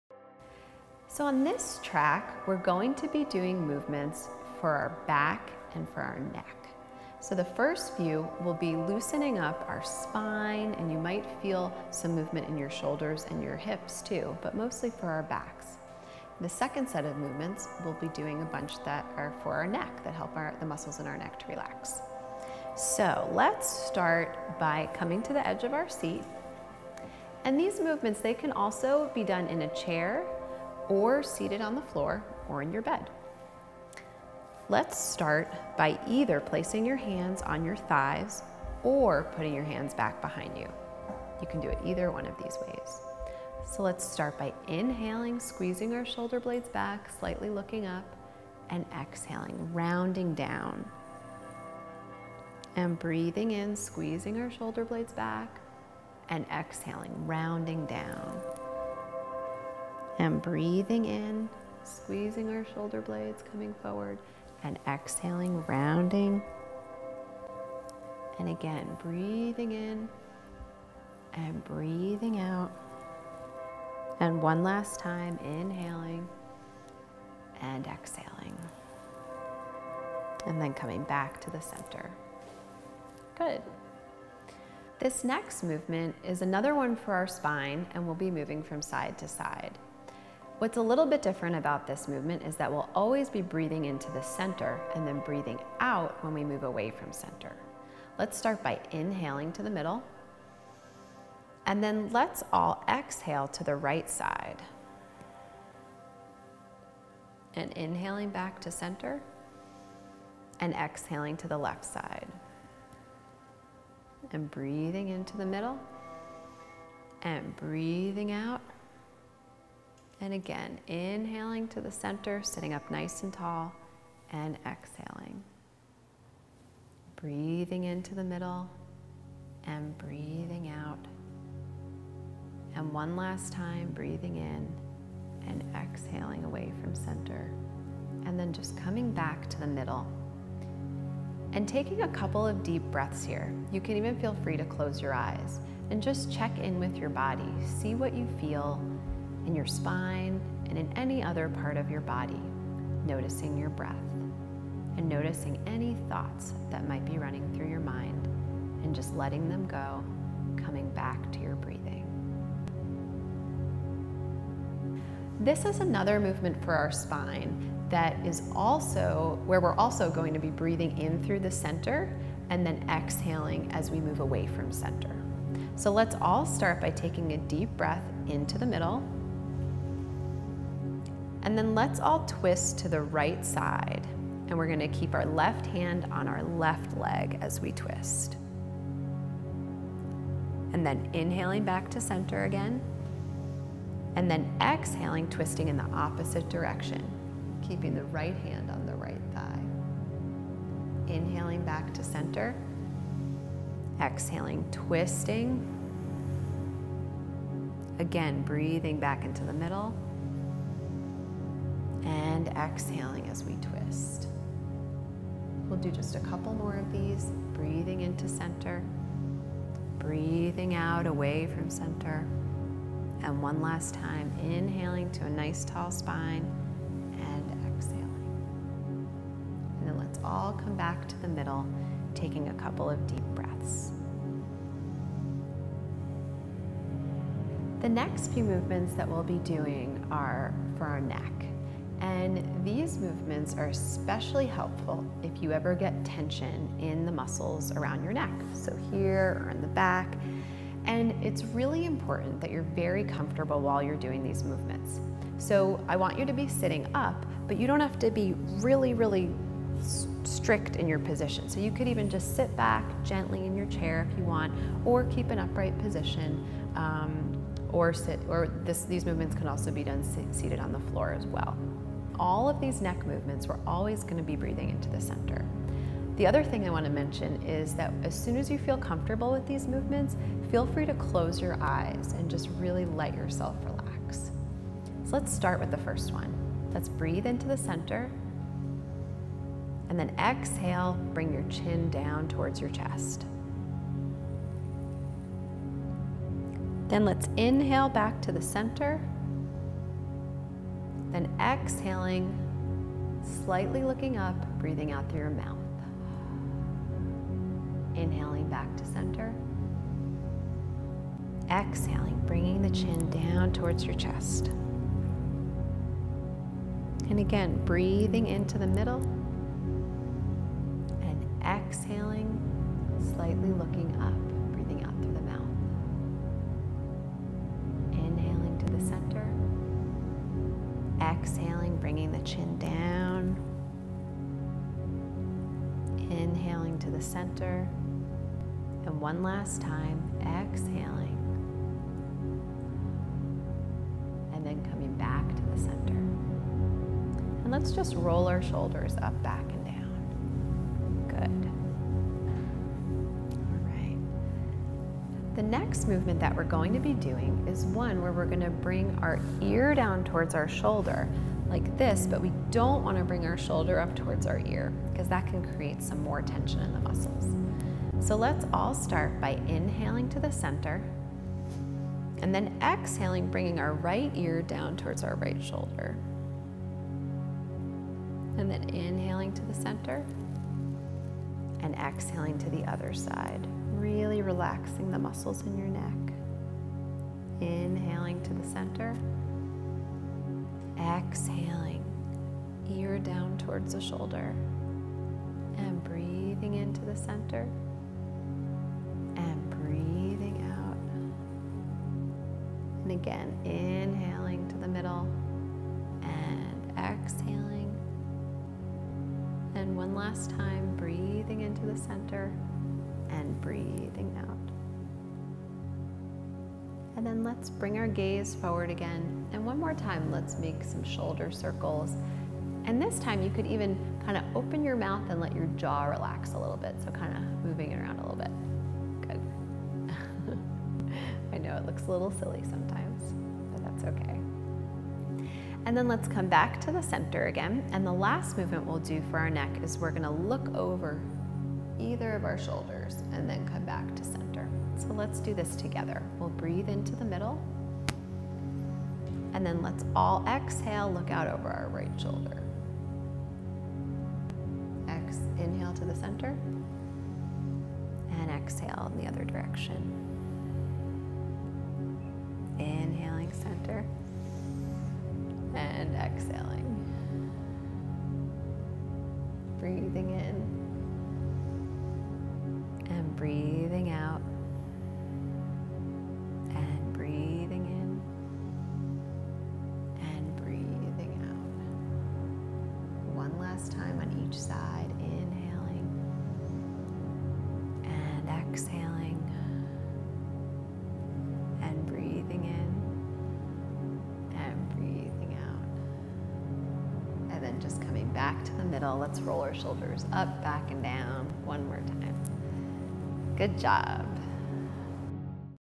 Yoga for Teens with Arthritis - Spine and Neck
This video focuses on gentle movements for the spine and neck based on the Joint-Freeing Series (TM). These movements can be done seated in a chair or on the floor.

The “Yoga for Teens with Arthritis,” video was produced in 2011 by Amanda Feinstein, PhD, as part of a research program on pain relief for youth with Juvenile Idiopathic Arthritis and was a collaboration between the Department of Pediatric Rheumatology at Emory University and the Department of Psychology at Georgia State University. Production of the film was funded by a grant awarded to Dr. Amanda Feinstein and Dr. Sampath Prahalad through the 2011/2012 Abbot Health Professional Graduate Student Preceptorship Award under the American College of Rheumatology Research and Education Foundation.

The yoga program displayed in the video is based on the Joint-Freeing Series (TM) by Mukunda Stiles and was adapted for adolescents with juvenile arthritis. The video contains movements for increasing range of motion in the joints and stretching/strengthening the muscles that support the joints.  Although this video was created specifically for youth with arthritis, adolescents and adults with other pain conditions may also benefit from it. Please talk with your physician or care team (e.g., physical or occupational therapist) to determine if this yoga sequence is appropriate for helping you before using this video.

Copyright Amanda Feinstein (2011)

Yoga Teacher and Narrator: Amanda Feinstein, PhD
Directed by: Amanda Feinstein and Jamie Olmstead
Cinematography and Editing: Jamie Olmstead
This video was part of a doctoral dissertation mentored by Drs. Lindsey Cohen and Akihiko Masuda